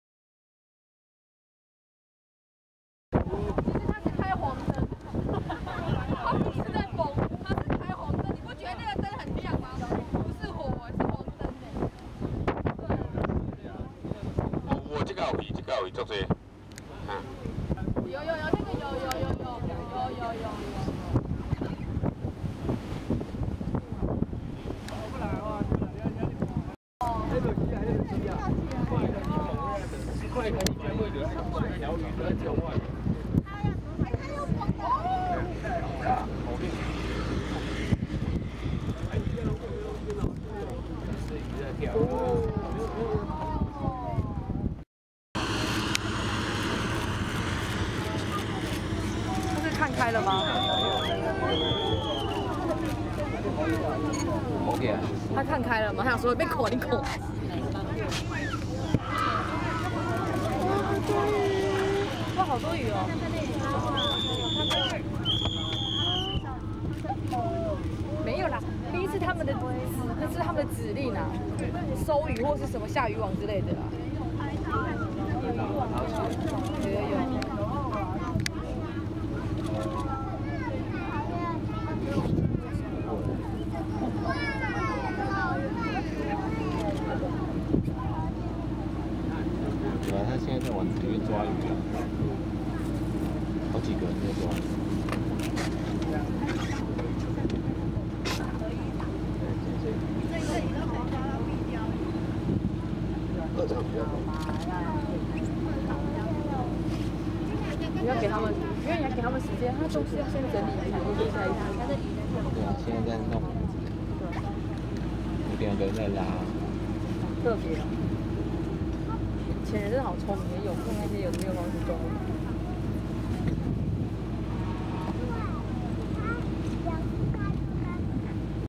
2014.6.22 基隆港外看蹦火仔
蹦火仔，磺火捕魚，是金山一帶獨特的傳統捕魚方式。漁民利用俗稱「電火石」的磺石，在泡水之後產生乙炔氣體再點火引燃，耀眼的磺火就能吸引具趨光性的「青鱗仔」魚群，也因為點火時會傳出類似爆炸的聲響，所以被稱做「蹦火仔」。